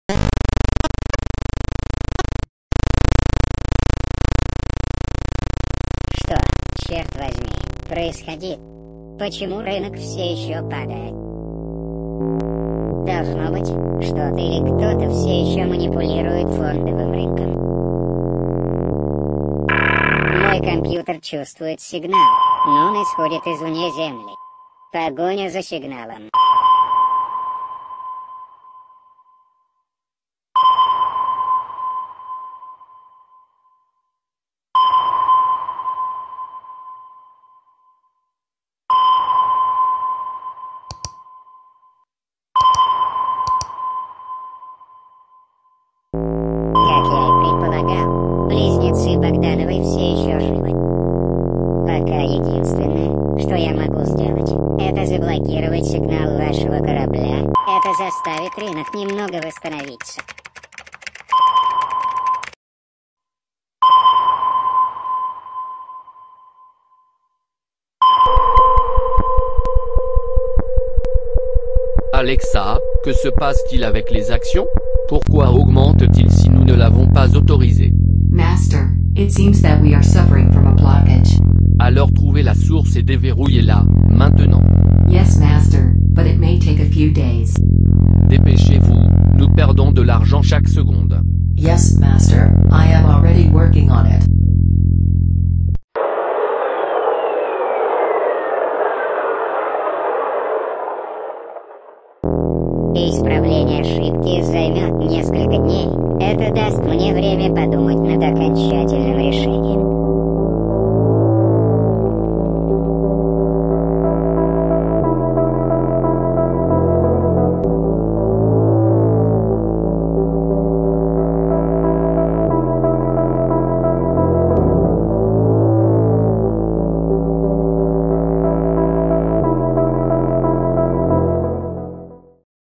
Sminem finds out about Bogdanoff's death
Sminem finds Bogdanoff twins conspiracy
In this episode, Sminem, who is in his bunker, perceives that something is affecting the stock market, thanks to his technology he is able to find the source of the problem.